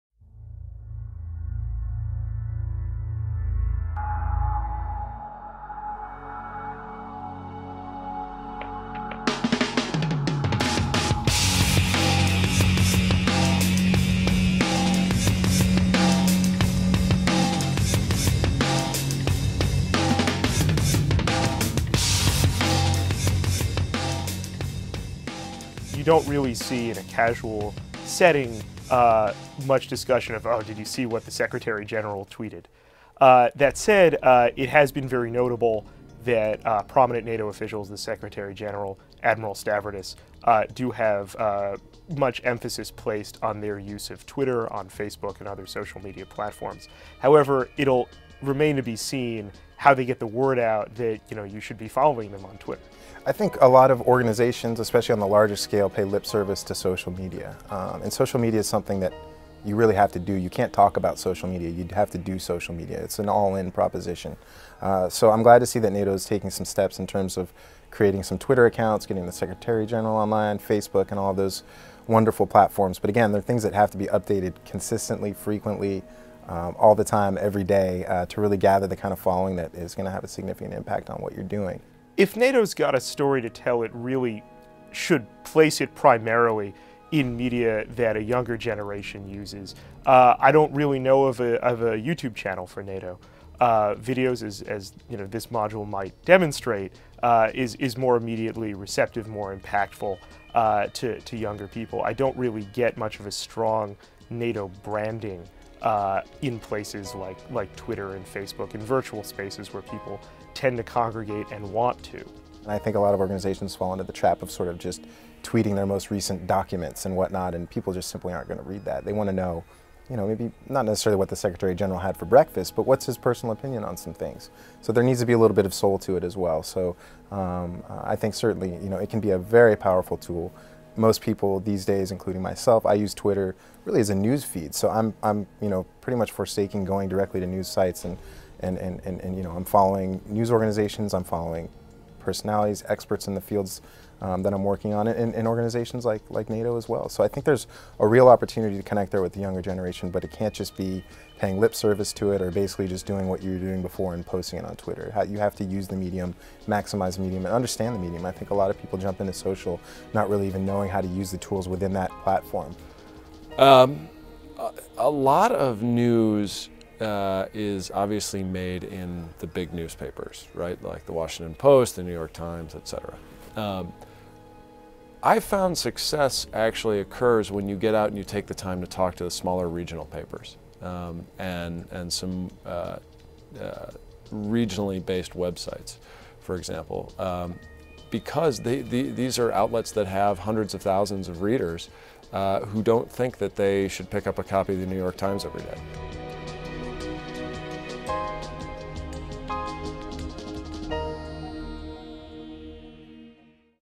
NATO Review - Social media and NATO: it's complicated
The US is the home of social media. Facebook, Twitter, YouTube and many others were born there. So could social media be the means that brings NATO closer to US people? We asked a number of communications experts what progress they feel has been made - and what more needs to be done. NATO Homepage